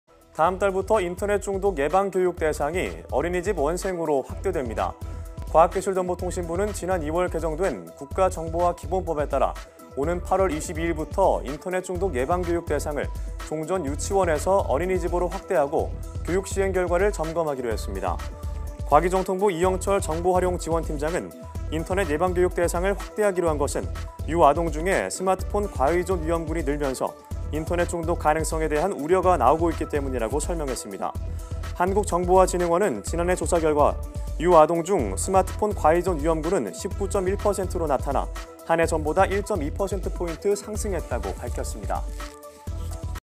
인터넷중독 예방 교육 어린이집도 포함 / YTN 사이언스
다음 달부터 인터넷 중독 예방 교육 대상이 어린이집 원생으로 확대됩니다.

과학기술정보통신부는 지난 2월 개정된 국가정보화기본법에 따라 오는 8월 22일부터 인터넷중독 예방 교육 대상을 종전 유치원에서 어린이집으로 확대하고, 교육 시행 결과를 점검하기로 했습니다.

과기정통부 이영철 정보활용지원팀장은 인터넷 예방교육 대상을 확대하기로 한 것은 유·아동 중에 스마트폰 과의존 위험군이 늘면서 인터넷 중독 가능성에 대한 우려가 나오고 있기 때문이라고 설명했습니다.

한국정보화진흥원은 지난해 조사 결과 유·아동 중 스마트폰 과의존 위험군은 19.1%로 나타나, 한 해 전보다 1.2%포인트 상승했다고 밝혔습니다.